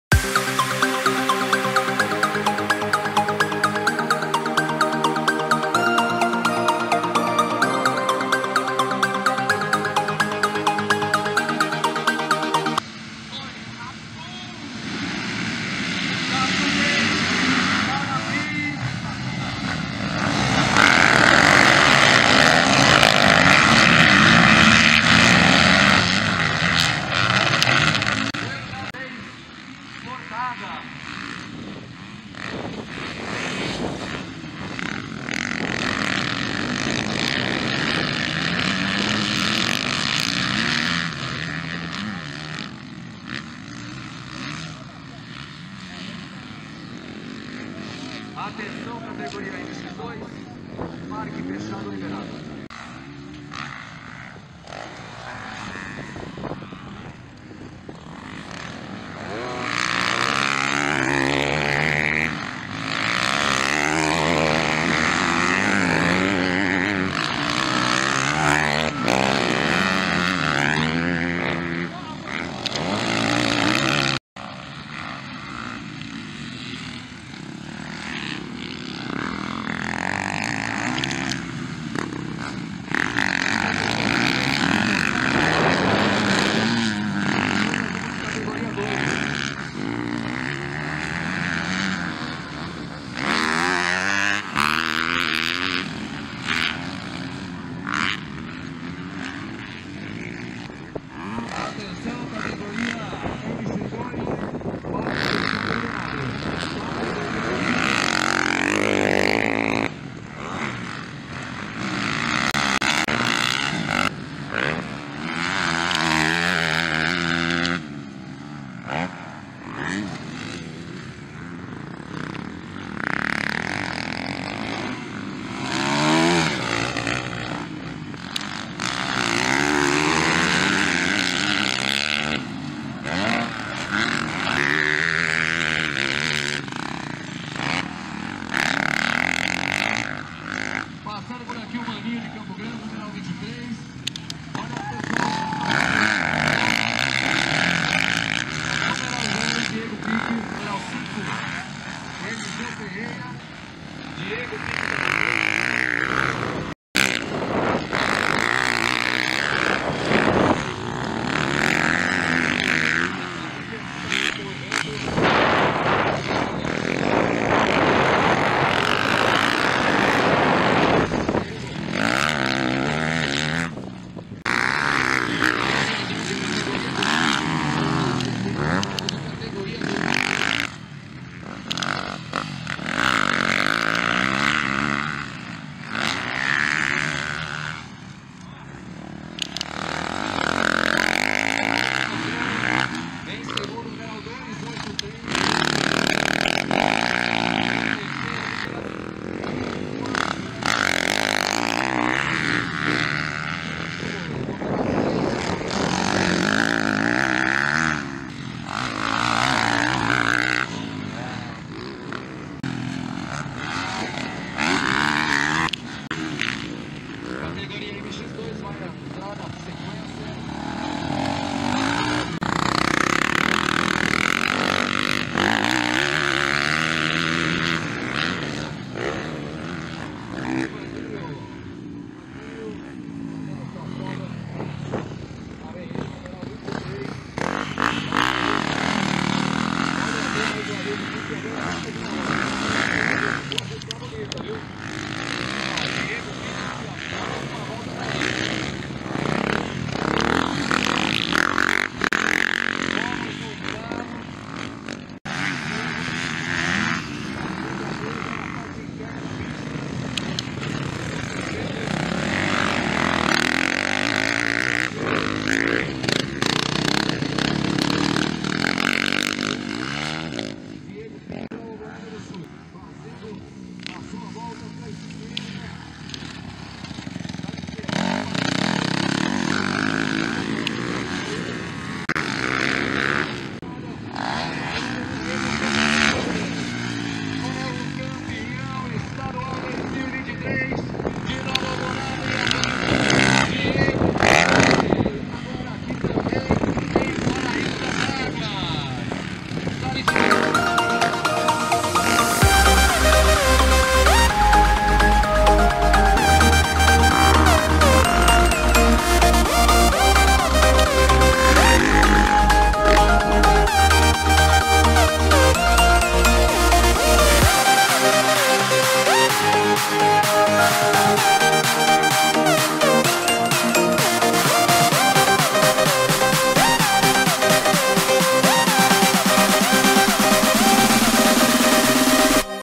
Pich domina MX3 na decisão do Sul-mato-grossense de Motocross 2023 em Paraíso das Águas
Bateria disputada no domingo (29) em Paraíso das Águas (MS).

Acompanhe o velocross e motocross Sul-mato-grossense no blog @TVHoleshot